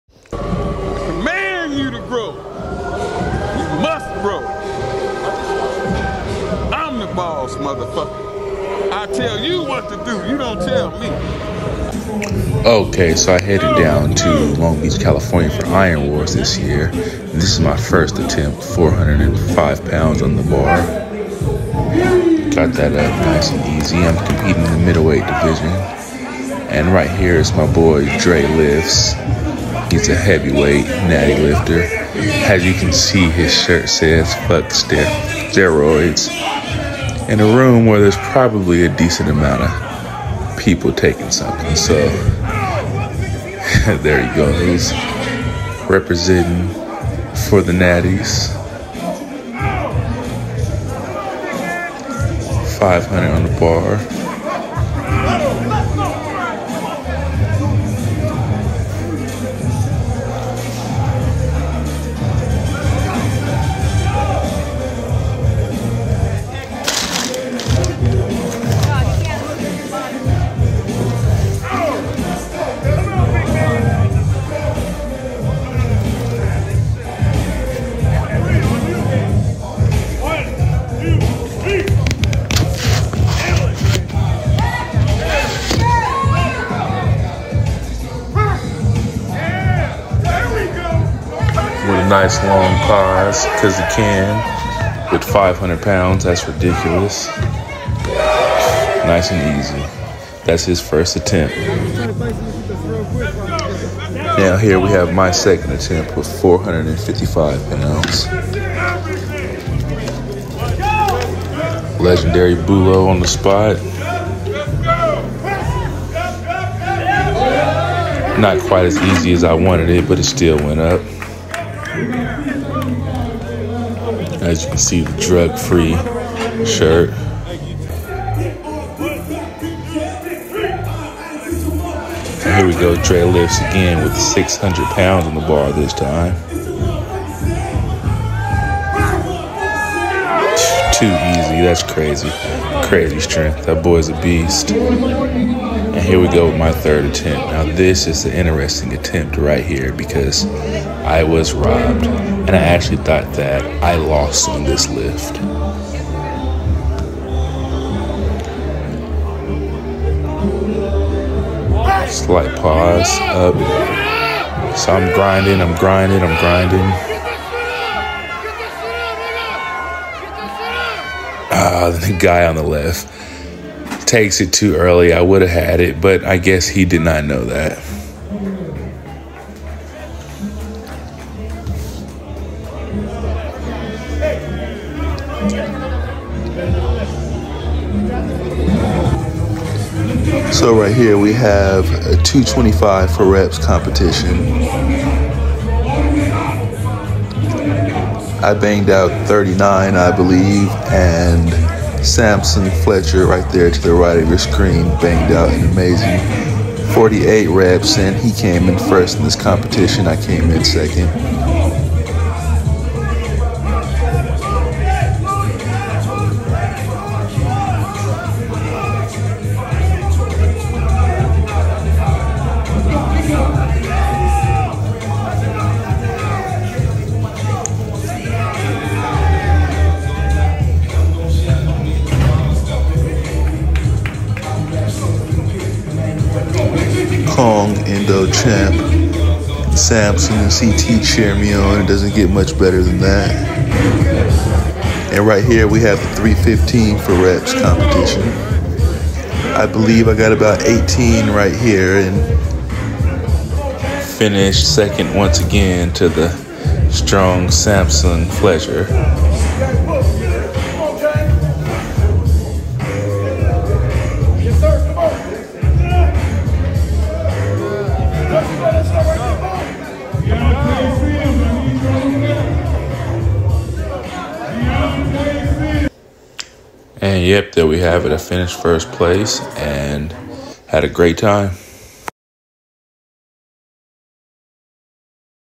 Iron Wars 6
My trip to Iron Wars 6 ( 1st place middle weight bench press)